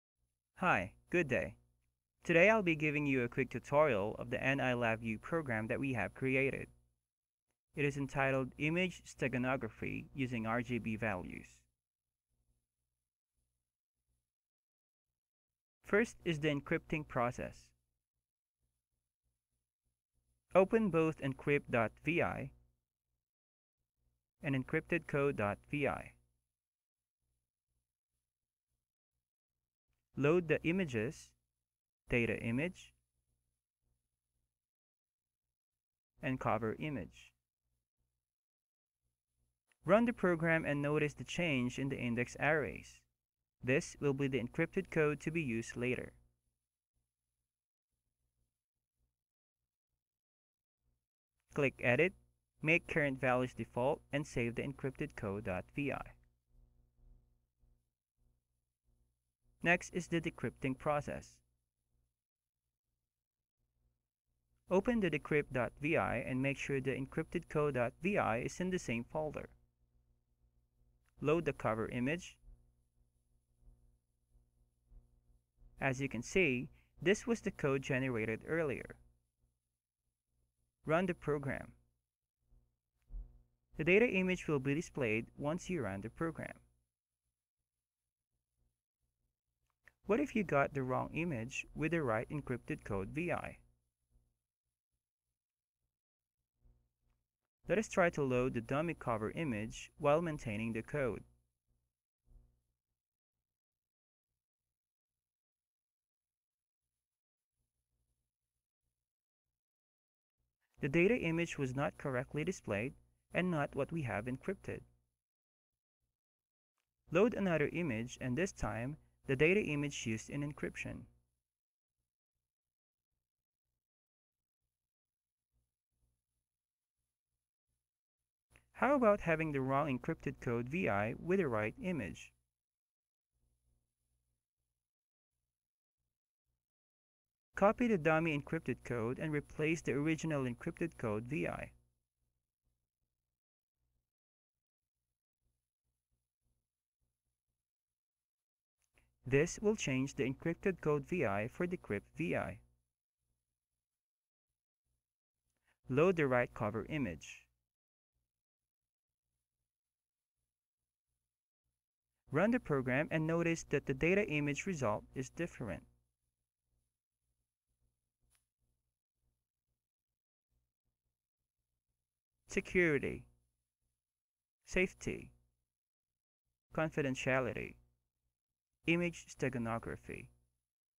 Image Steganography Using RGB Values
A video submitted by Caguimbal, R.R., Guanlao, M.M., Montano, J.C., Yasay, A.L. and Fernando, A.R., Technological Institute of the Philippines for National Instruments - Graphical System Design Achievement Awards 2013.